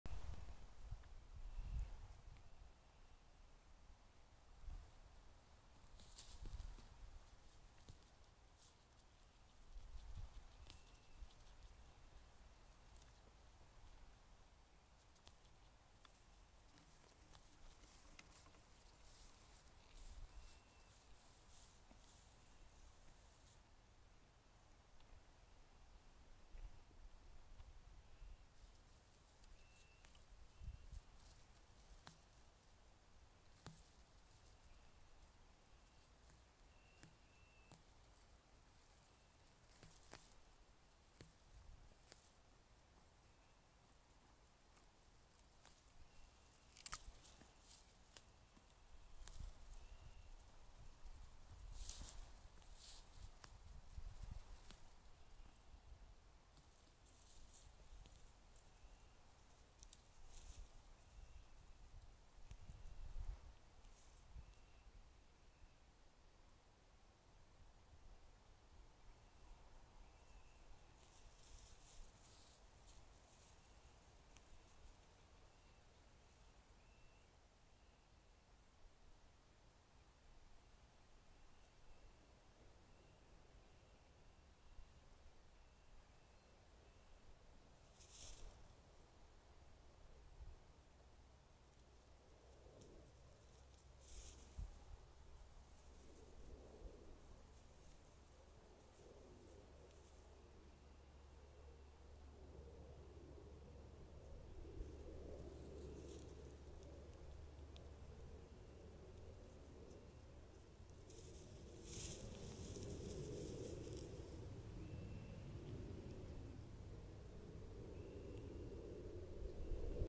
Deer Diary: Can you get any closer?
This deer really couldn't get much closer to the camera!